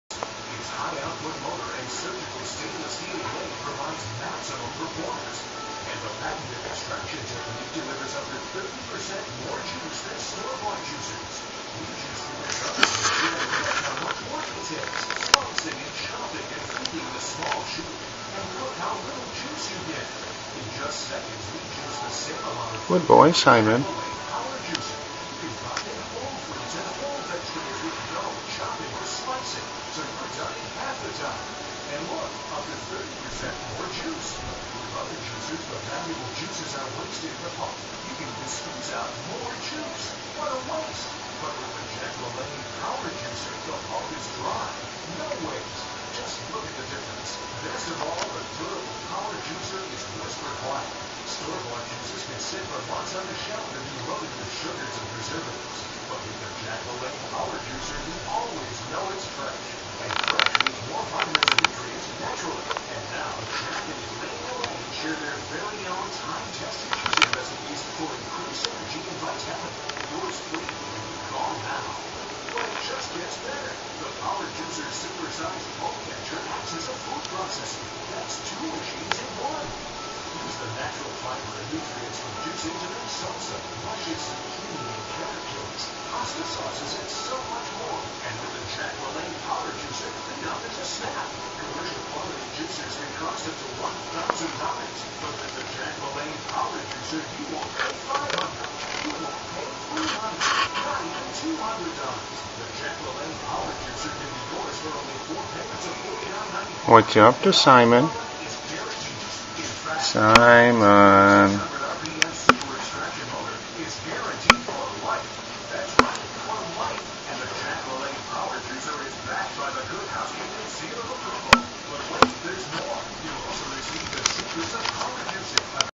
Simon eating Salmon on Christmas
Simon eating Fancy Feast salmon on Christmas morning.